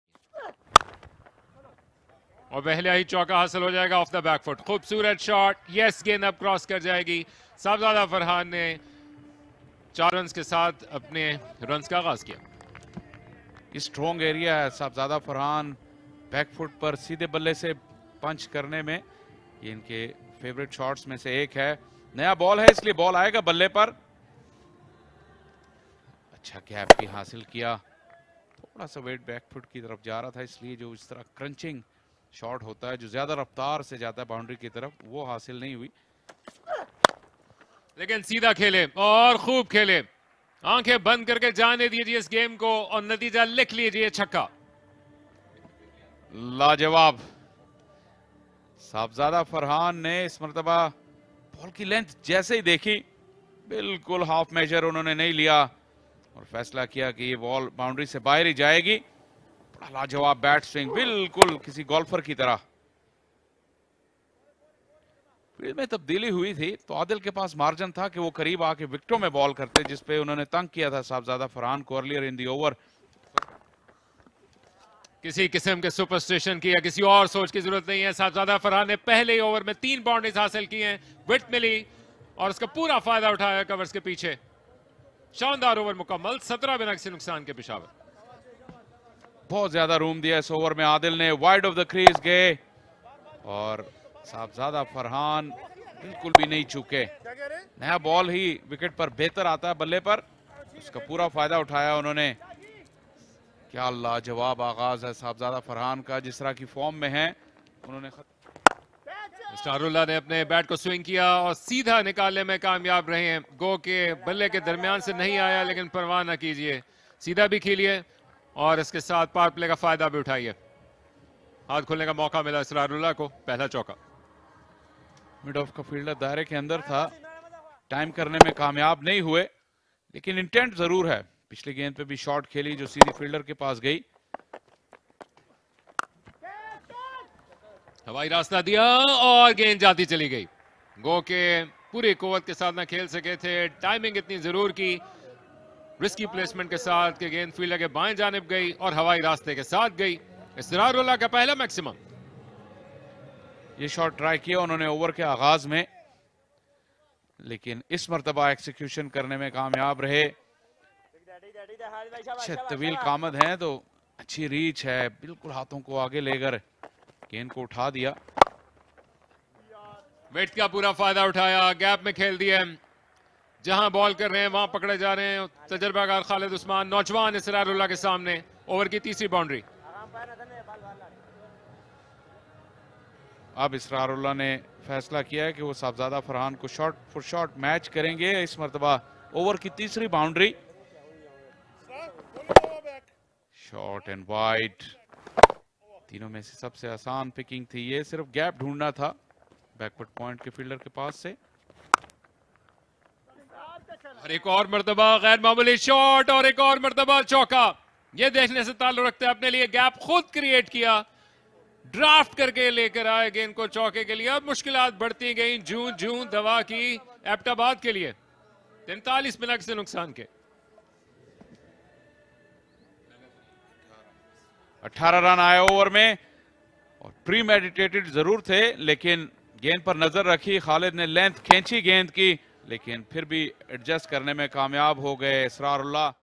Unstoppable Shots | Peshawar on Fire | Peshawar vs Abbottabad | National T20 2023-24 | PCB | M1W1A
Unstoppable Shots | Peshawar on Fire | Peshawar vs Abbottabad | Match 51 | National T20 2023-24 | PCB | M1W1A